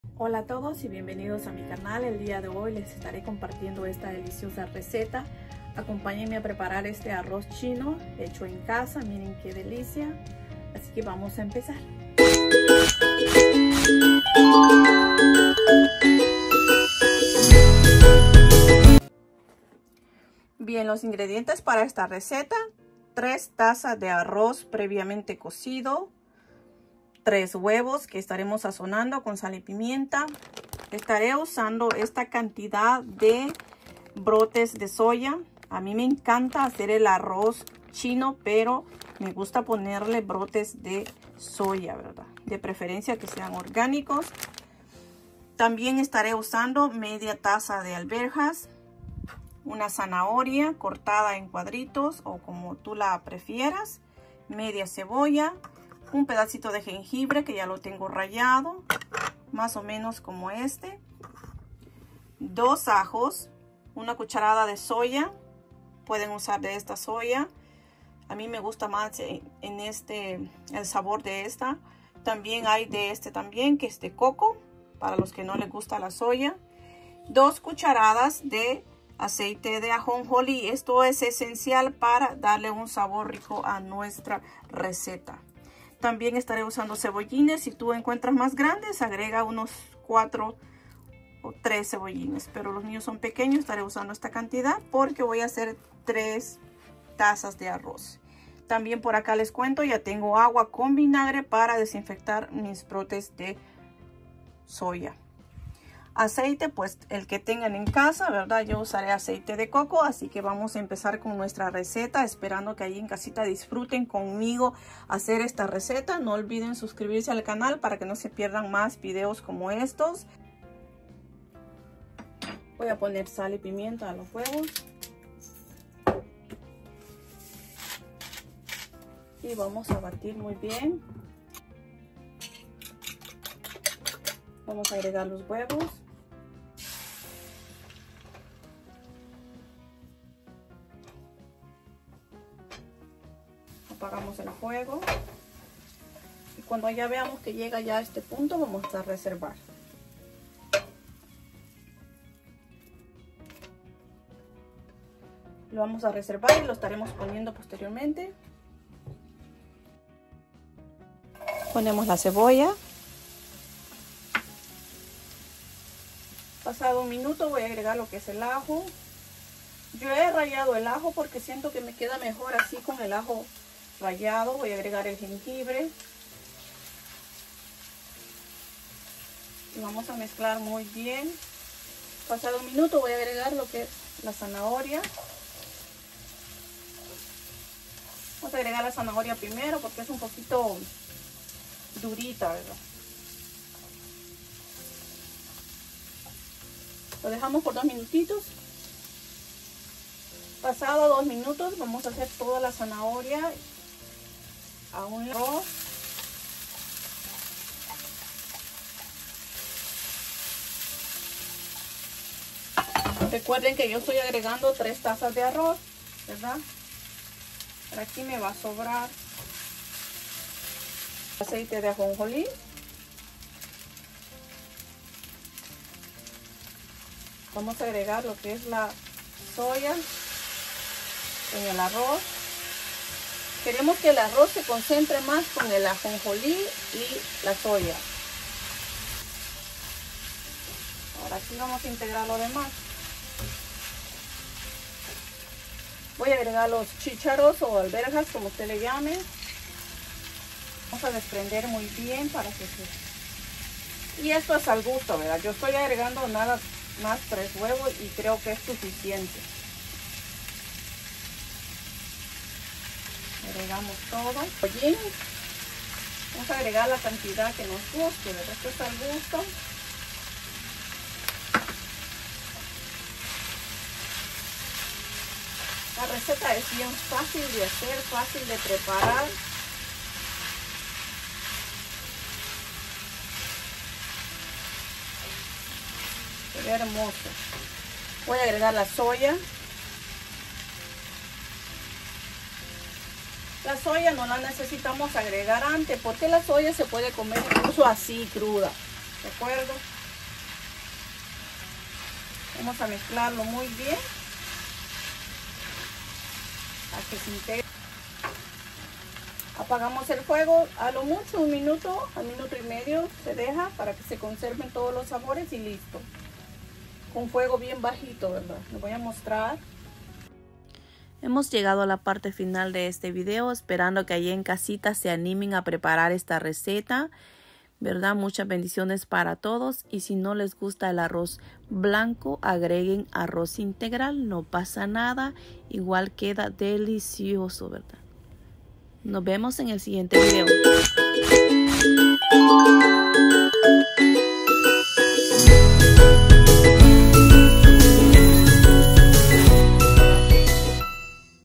RECETA DE ARROZ CHINO CON BROTES DE SOJA 🌱🍽️
Una deliciosa receta para toda la familia

Ingredientes
3 tazas de arroz cocido 🍲
3 huevos sazonar con sal y pimienta al gusto
7 Onzas de Soja = 198gramos
1/2 taza de alverjas
1 zanahoria 🥕 picada en cuadros pequeños
1/2 cebolla 🧅
1/4 de jengibre 🫚 rallado
2 ajos 🧄 rallados
1-2 cucharadas se salsa de Soja
2 cucharadas de aceite de ajonjolí
1/2 taza de cebollin picado
Y una cucharada de aceite de coco 🥥
O el de tu preferencia